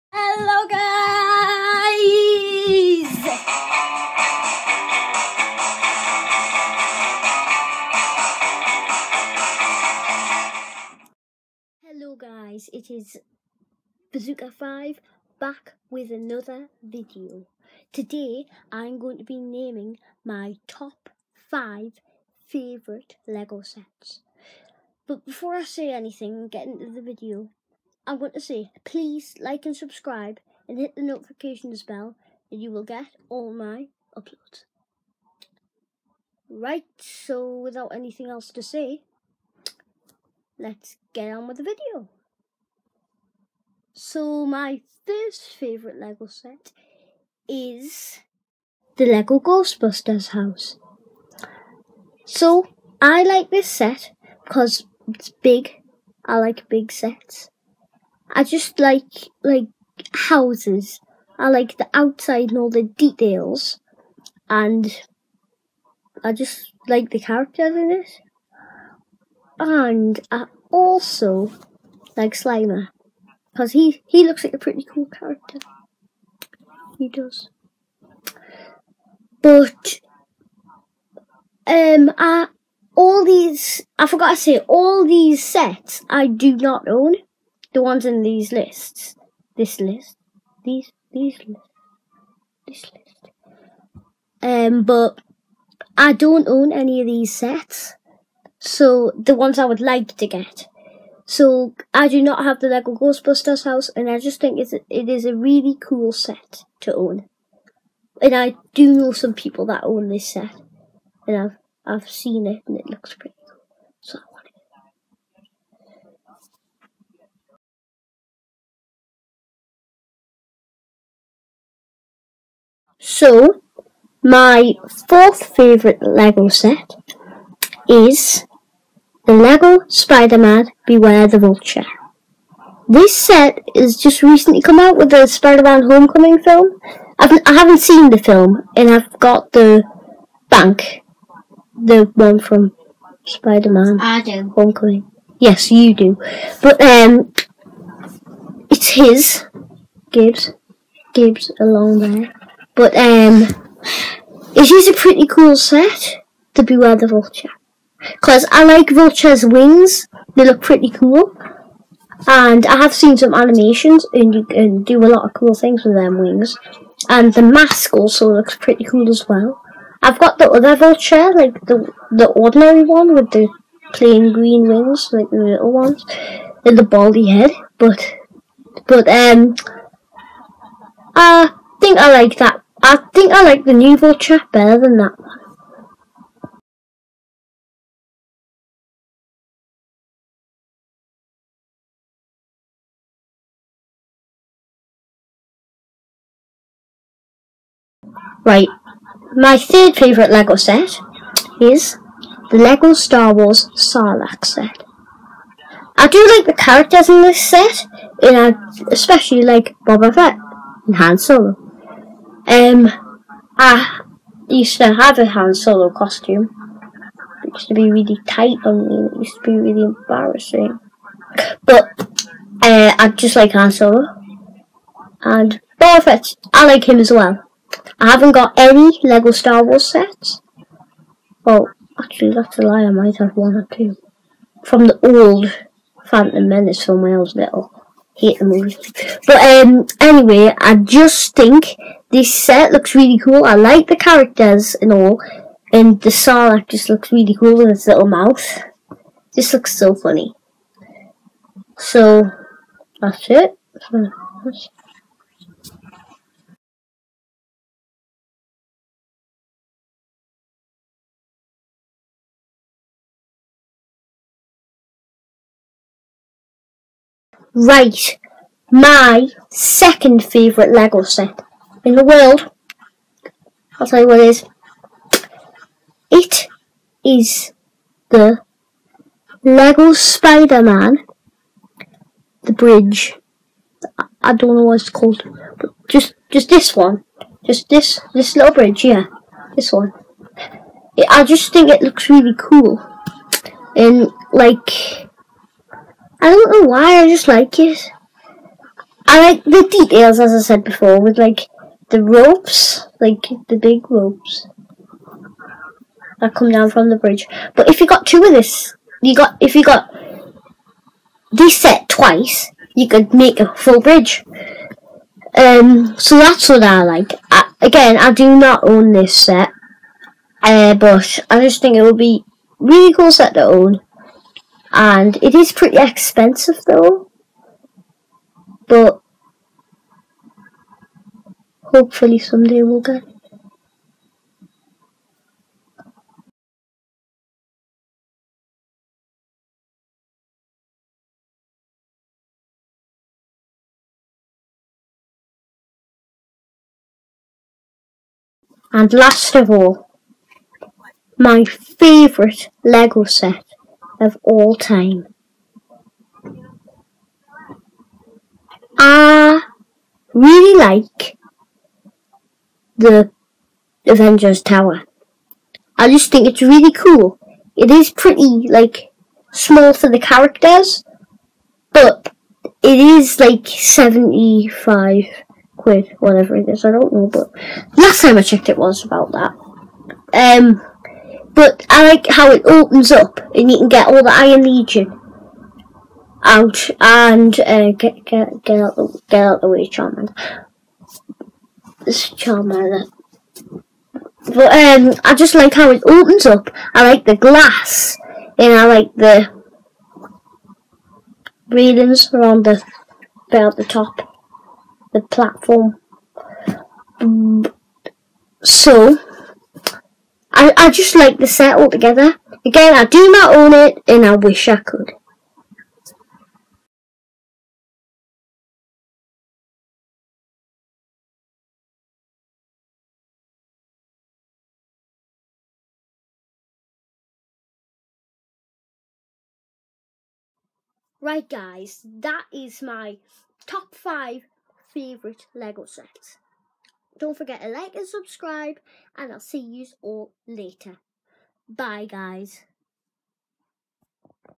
My top 5 Lego sets
I name my top 5 Lego sets